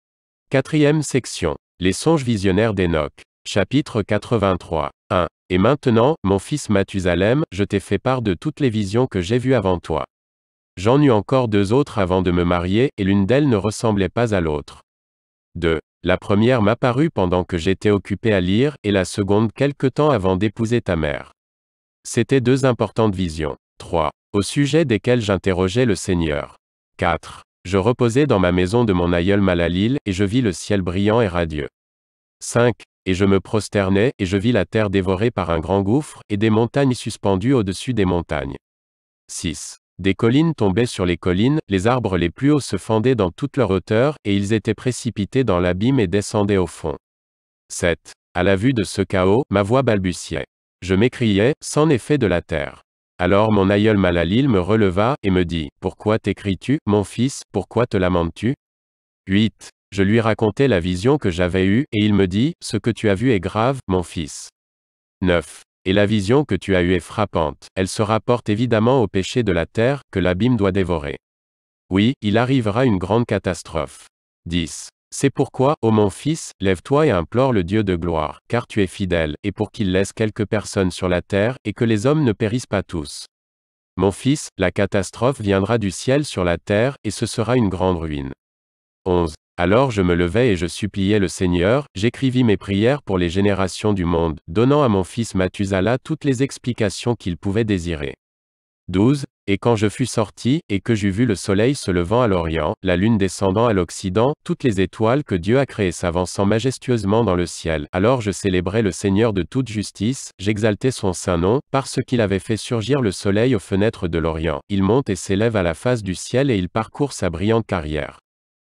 La Bible Audio en Français : Le Livre d'Énoch - Chapitre 83
1. Et maintenant, mon fils Mathusalem, je t'ai fait part de toutes les visions que j'ai vues avant toi. J'en eus encore deux autres avant de me marier, et l'une d'elles ne ressemblait pas à l'autre.
2. La première m'apparut pendant que j'étais occupé à lire; et la seconde quelque temps avant d'épouser ta mère. C'étaient deux importantes visions.
3. Au sujet desquelles j'interrogeai le Seigneur.

4. Je reposais dans ma maison de mon aïeul Malaleel, et je vis le ciel brillant et radieux.
5. Et je me prosternai, et je vis la terre dévorée par un grand gouffre, et des montagnes suspendues au-dessus des montagnes.
6. Des collines tombaient sur les collines, les arbres les plus hauts se fendaient dans toute leur hauteur, et ils étaient précipités dans l'abîme et descendaient au fond.
7. A la vue de ce chaos, ma voix balbutiait. Je m'écriai: C'en est fait de la terre. Alors mon aïeul Malaleel me releva, et me dit: Pourquoi t'écries-tu, mon fils, pourquoi te lamentes-tu?
8. Je lui racontai la vision que j'avais eue, et il me dit: Ce que tu as vu est grave, mon fils.
9. Et la vision que tu as eue est frappante; elle se rapporte évidemment aux péchés de la terre, que l'abîme doit dévorer. Oui, il arrivera une grande catastrophe.
10. C'est pourquoi, ô mon fils, lève-toi et implore le Dieu de gloire, car tu es fidèle, et pour qu'il laisse quelques personnes sur la terre, et que les hommes ne périssent pas tous. Mon fils, la catastrophe viendra du ciel sur la terre; et ce sera une grande ruine.
11. Alors je me levai et je suppliai le Seigneur; j'écrivis mes prières pour les générations du monde, donnant à mon fils Mathusala toutes les explications qu'il pouvait désirer.
12. Et quand je fus sorti, et que j'eus vu le soleil se levant à l'orient, la lune descendant à l'occident, toutes les étoiles que Dieu a créées s'avançant majestueusement dans le ciel, alors je célébrai le Seigneur de toute justice, j'exaltai son Saint Nom, parce qu'il avait fait surgir le soleil aux fenêtres de l'orient; il monte et s'élève à la face du ciel et il parcourt sa brillante carrière.